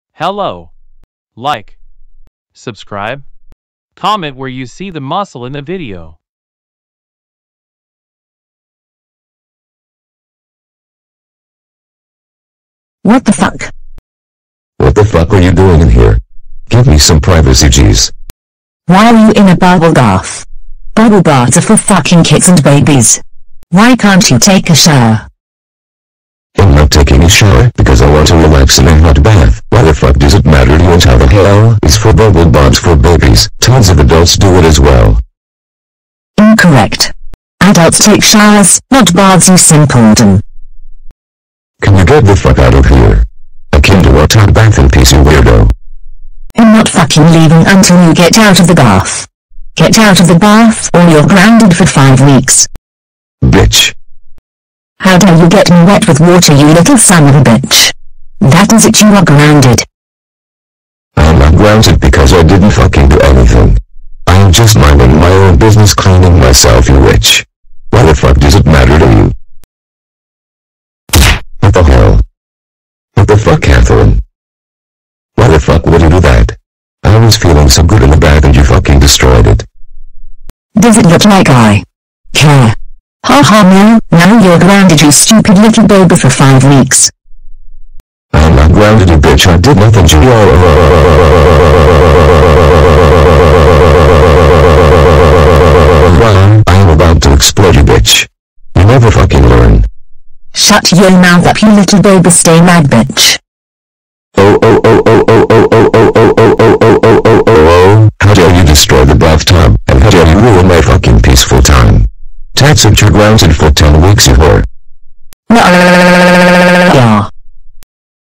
Childish Dad Takes A Bubble Bath
Childish dad (dave) wanted to unwind taking a nice hot bubble bath, but catherine for whatever reason came in the bathroom saying how bubble baths are for kids and babies.

Title: Childish Dad Takes A Bubble Bath

CHILDISH DAD WAS ORIGINALLY MADE BY THERANDOMLOZZIE.